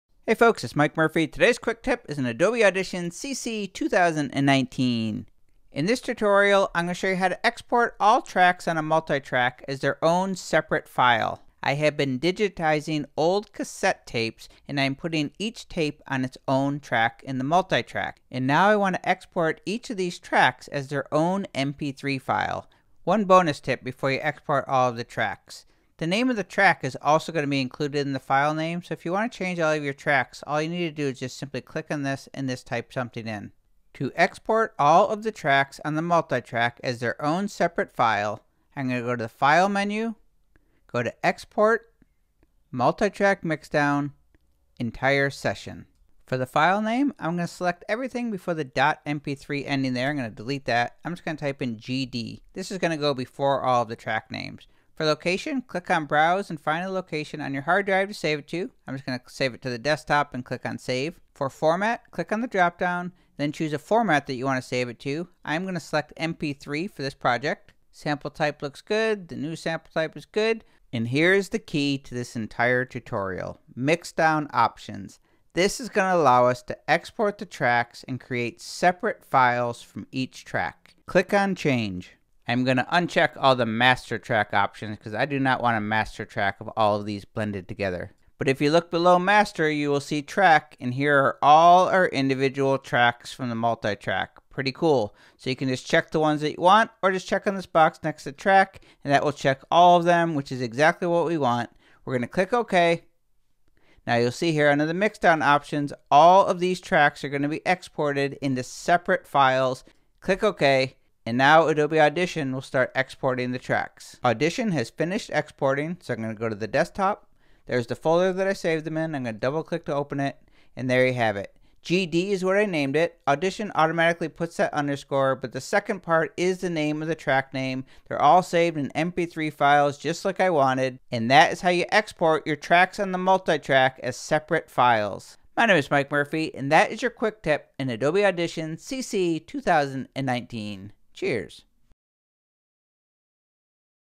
Adobe Audition: Export Tracks To Separate Files in Multitrack
How To Export Tracks in Multitrack to Separate Files in Adobe Audition CC 2019

Adobe Audition CC 2019:
This tutorial will show you how to export some or all of the tracks in a  Multitrack Session to separate files (instead of a mixdown).
For example, you can export each track as their own mp3 file.

How To Export Tracks As Separate Files in Adobe Audition CC 2019:
1. Open up a project in the Multitrack of Adobe Audition (multiple tracks of audio)
2. To Rename Tracks…Click on the Track Title on the left side of track and type in new name
3. Go to File…Export…Multitrack Mixdown….Entire Session
4. File name is the ‘Prefix’ before the Track Name (Keep the file name extension such as .mp3, but you can delete everything else if you only want the exported files to be the name of the Track. Audition will always add an underscore (_) before the Track Name)
5. Click ‘Browse’ to choose a Location to Save
6. Click ‘drop-down’ arrow to change Format
7. Click ‘Change’ to change Sample Type
8. Click ‘Change’ to change Format Settings
9. ***Mixdown Options***Click ‘Change’
10. Uncheck the boxes under ‘Master’
11. Check the box next to ‘Track’ if you want to export all tracks as separate files or check individual tracks if you only want to export select tracks.
12. Click OK and Audition will export each track (can take a couple of minutes depending on how many tracks you have)
13. Check the Folder you saved to and all of your tracks should be there in the File Format you chose with the Prefix_Track Name